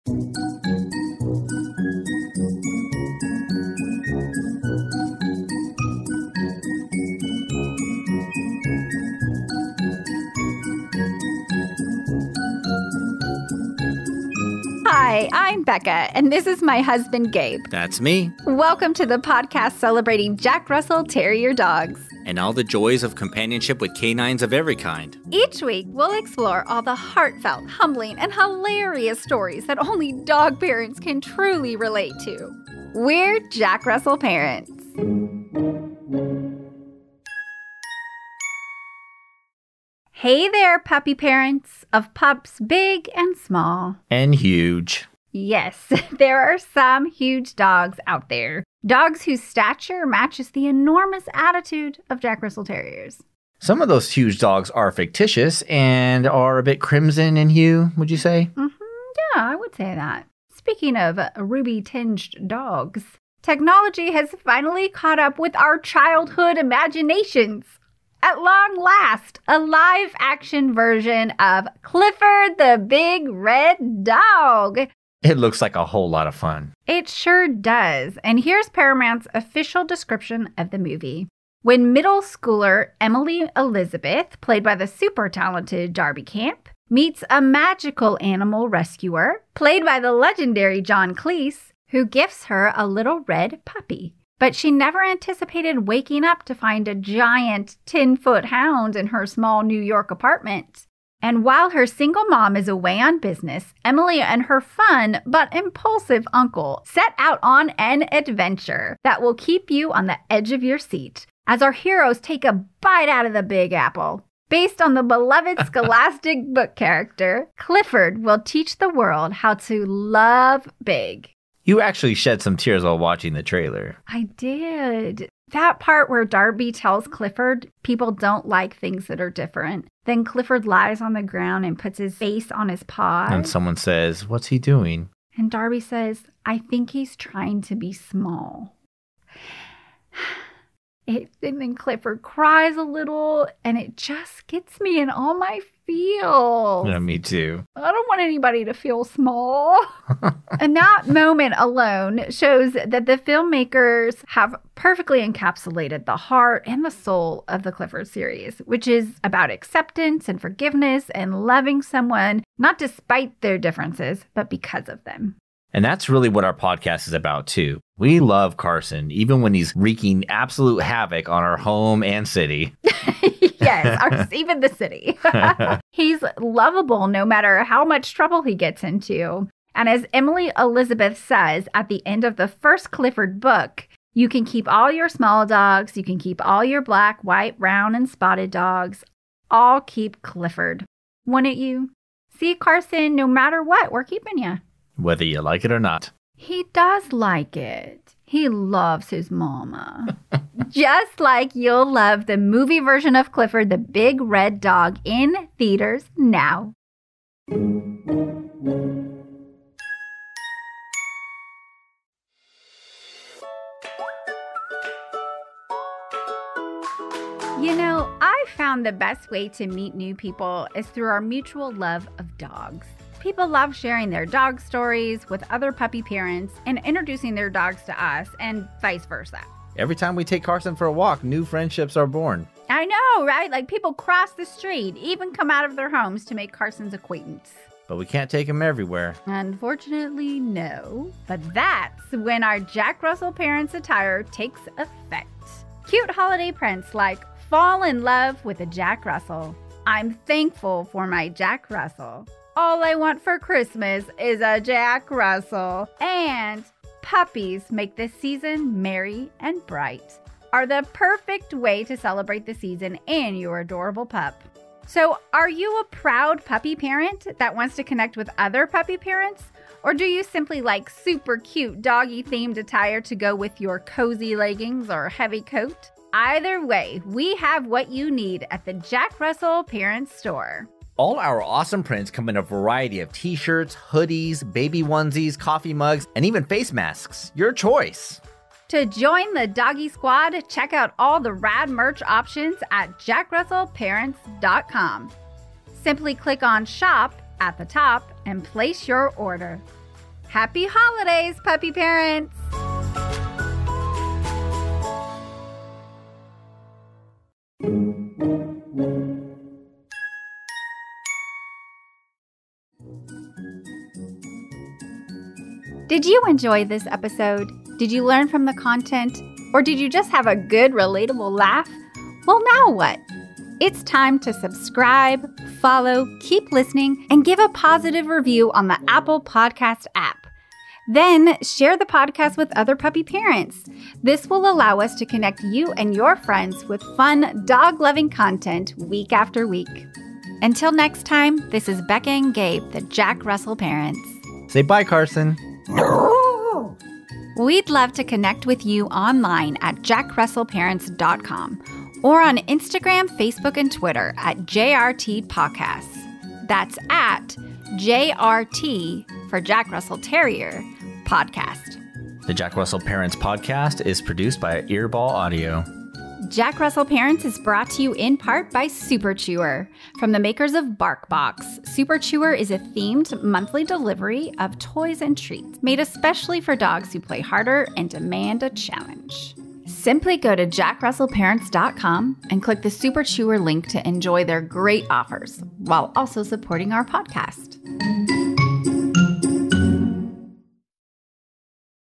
Clifford the Big Red Dog is a Hugely Adorable Tale! (Zoomiesode)
Technology has finally caught up to our childhood imaginations and at long last, a live-action version of Clifford the Big Red Dog hits theaters. Based upon Norman Bridwell's children's book series. Starring Darby Camp, Jack Whitehall, Izaac Wang, John Cleese, David Alan Grier, Kenan Thompson, Rosie Perez, and Sienna Guillory.